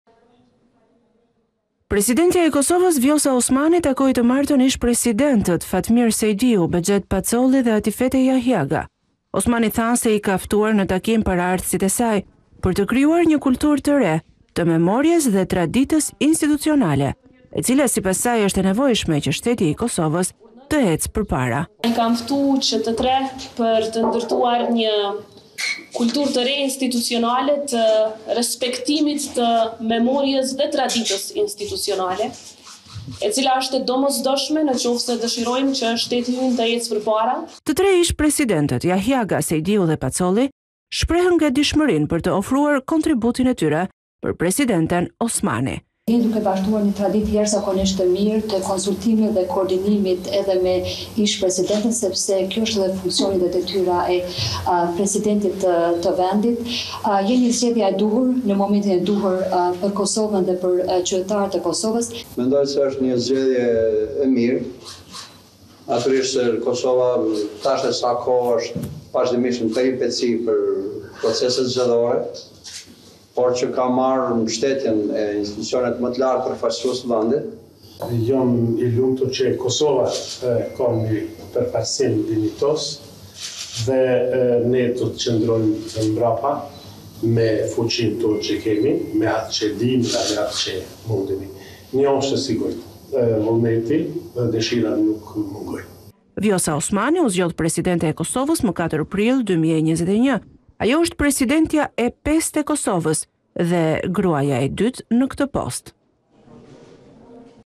Top News - Osmani takon ish presidentët/ ’Të krijojmë memorie dhe traditë të re’
Presidentja e Kosovës, Vjosa Osmani, takoi të martën ish-presidentët e Kosovës, Fatmir Sejdiu, Behgjet Pacolli dhe Atifete Jahjaga. Osmani tha se i ka ftuar në takim paraardhësit e saj, për të krijuar një kulturë të re “të memories dhe traditës institucionale”, e cila, sipas saj, është e nevojshme që shteti i Kosovës të ecë përpara.